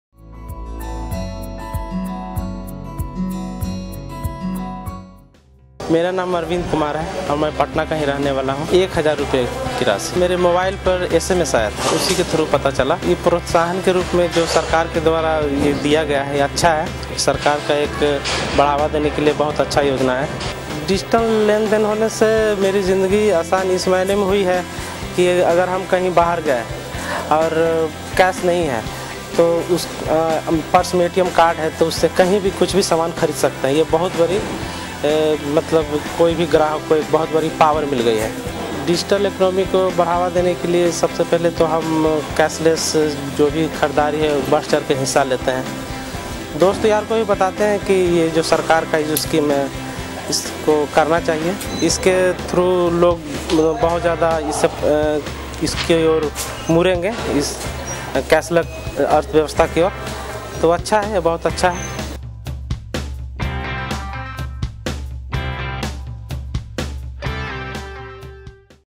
Lucky Grahak Yojana Winner, Arvind Kumar on Going Cashless
Arvind Kumar of Pakri village (Patna) wins cash reward in Lucky Grahak Yojana.

DigiDhan Vyapar Yojana and Lucky Grahak Yojana reward citizens to go cashless by using digital payments methods like RuPay Card, USSD Codes, UPI Apps and Aadhaar Enabled Payment System.